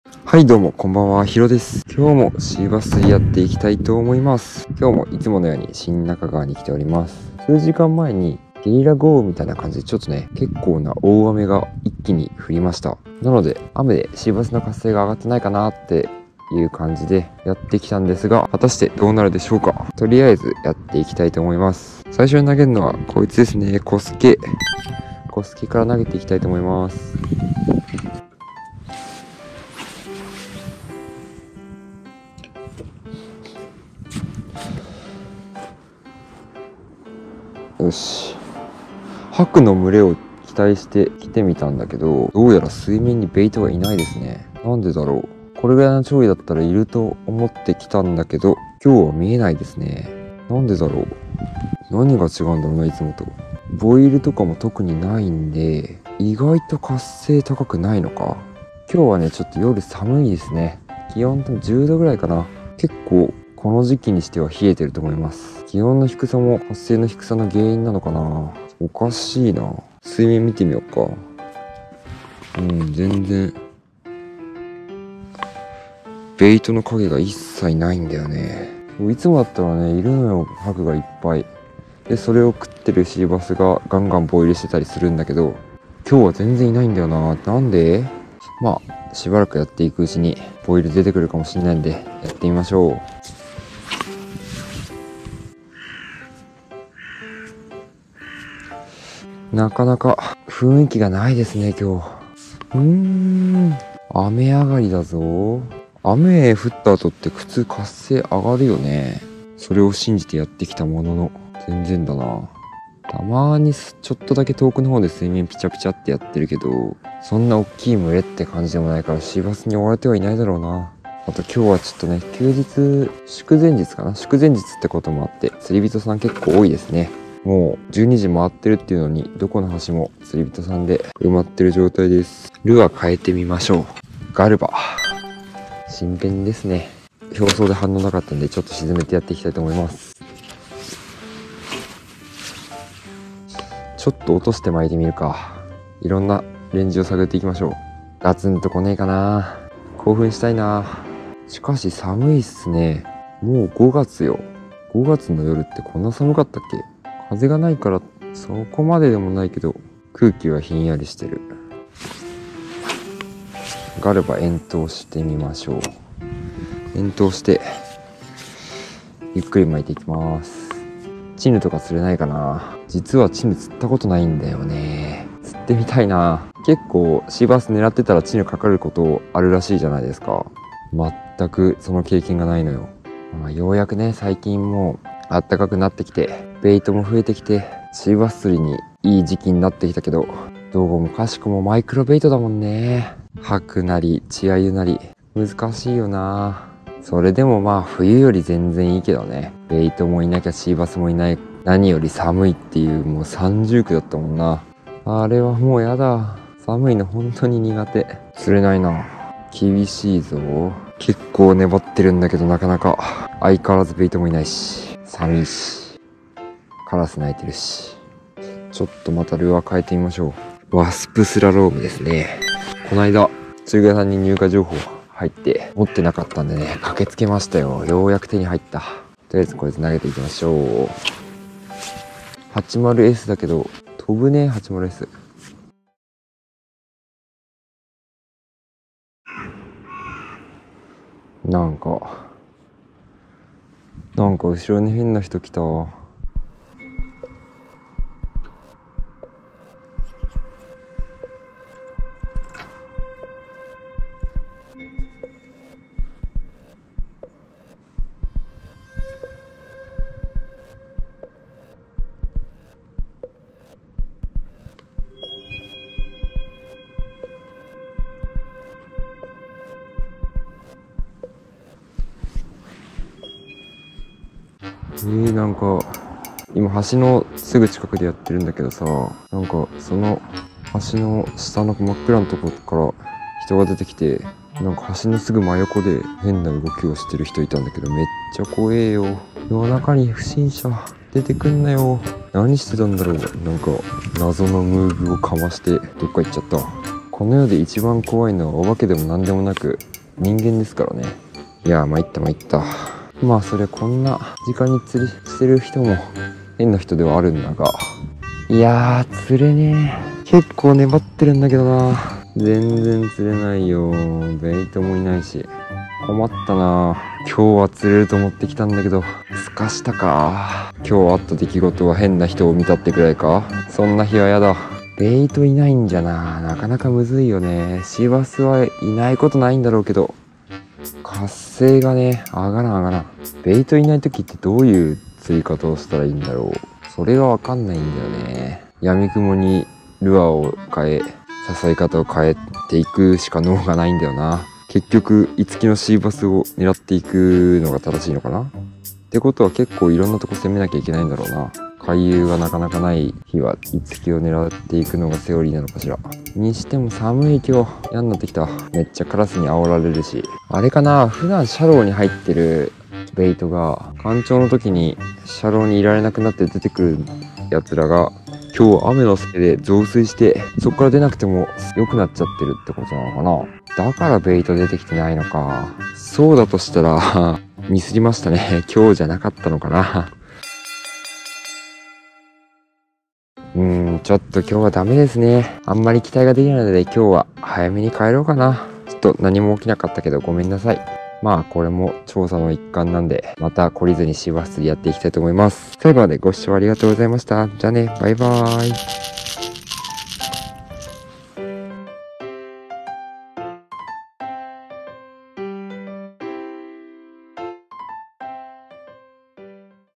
【都内シーバス】雨が降った後は魚が釣れるって聞いてたんで・・・
どこかの誰かが言ってました。
「雨上がりはシーバスが爆釣するよ」
素直な僕はその言葉を信じて、夜の川に繰り出すのだった。

東京湾奥　シーバス